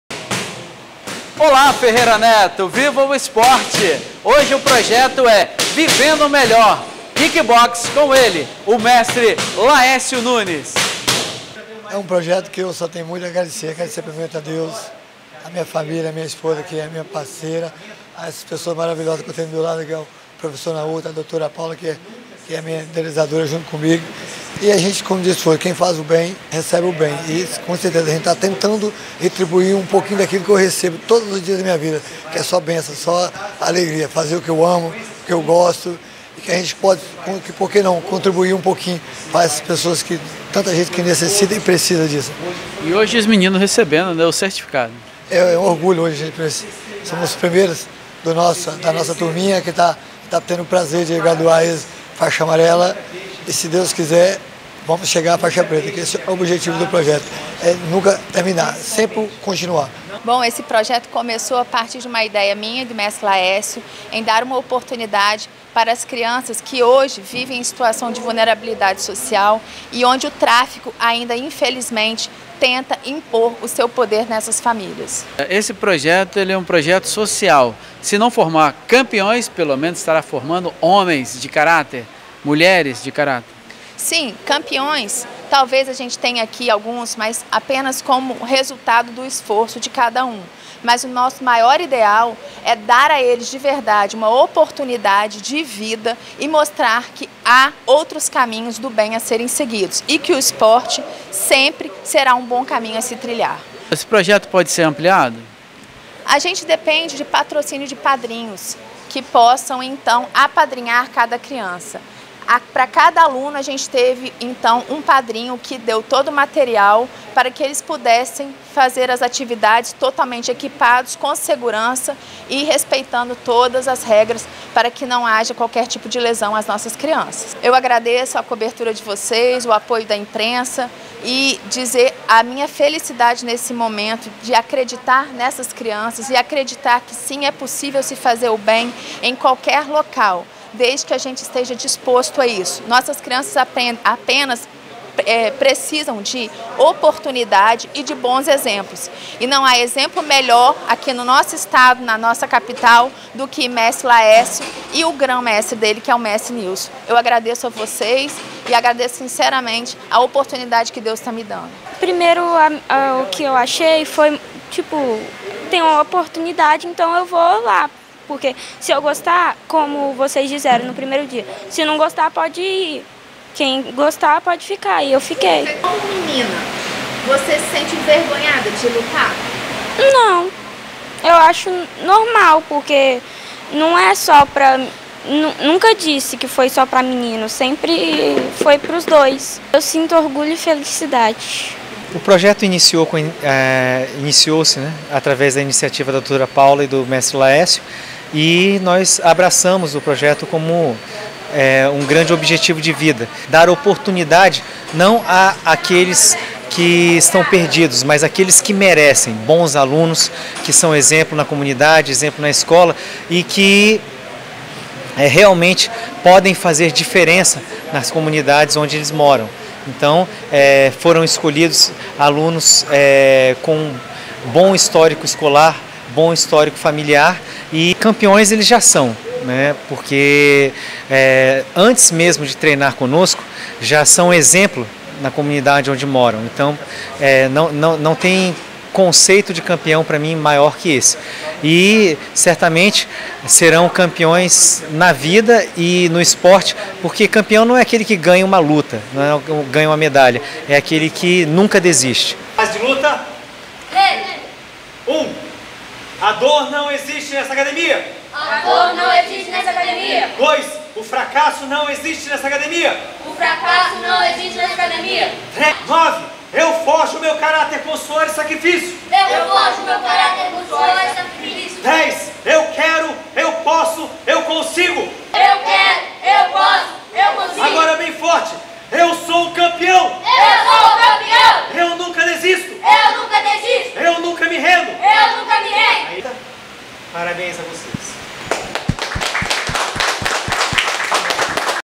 projeto social VIVENDO MELHOR.
iniciativa do Mestre Laécio Nunes e da Dra Paula Pazolini que visa premiar cidadãos de bem que moram em comunidades carentes, dando-lhes a oportunidade de mudança de vida através do FULL CONTACT.
Para ter direito a participar do projeto a criança ou o adolescente tem que estar matriculado na escola, ter boas notas e ser um aluno exemplar.
@Laecio Nunes @Paula pazoline @Naotohinode